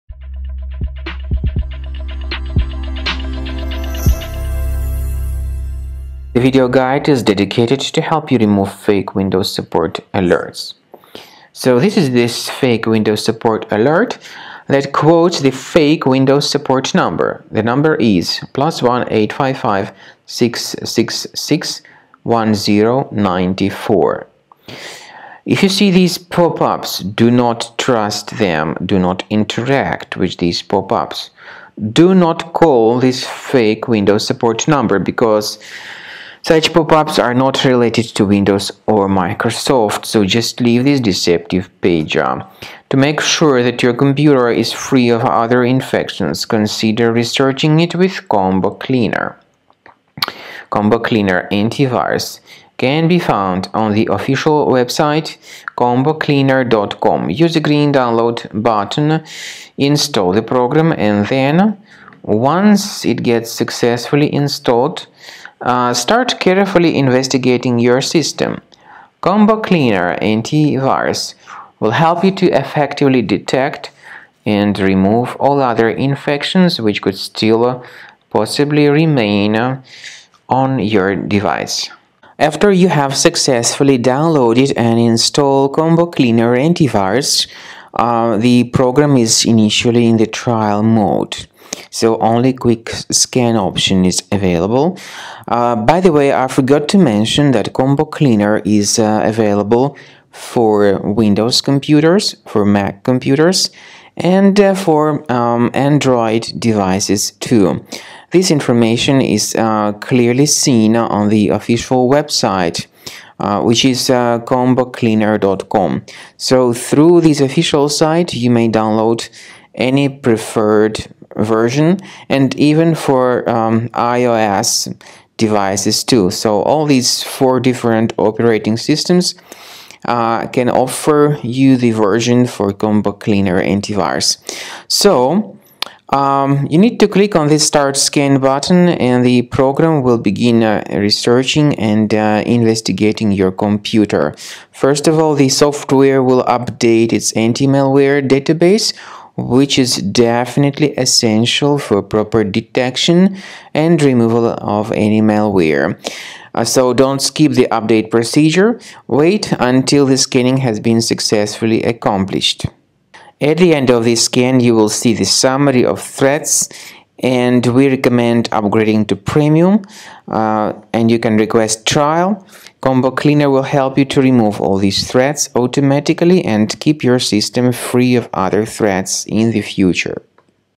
fake Windows Support alert removal.
It is mentioned in fake Windows Defender Security Center alerts. Don't interact with such pop-ups. Remove them with Combo Cleaner Antivirus via the official or, optionally, follow the manual tips given in the video to get your computer fixed.